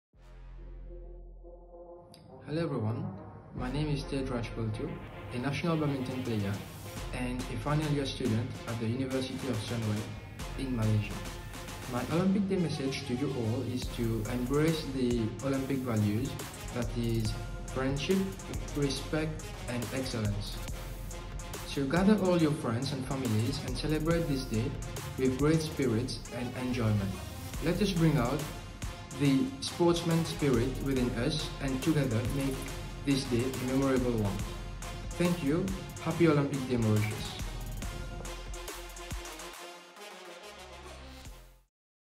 Tejraj Pultoo : «Let’s embrace the olympic values»
The National Badminton Team Player has a message for the Olympic Day, which will be celebrated on June 23.

“Let’s embrace the Olympic values, that are friendship, respect and excellence. Gather your families and friends to celebrate this day with great spirit and enjoyment. Let's bring out the sportsman spirit within us and make this day a memorable one. Happy Olympic Day Mauritius.»